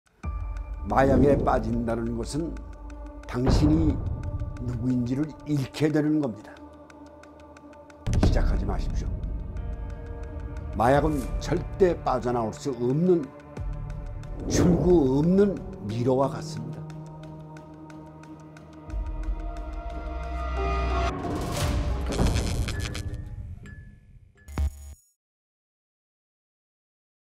NO EXIT 청소년 마약 예방 캠페인(경찰청)ㅣ 경기도교육청TV
출구 없는 미로 NO EXIT
마약 절대 시작하지 마세요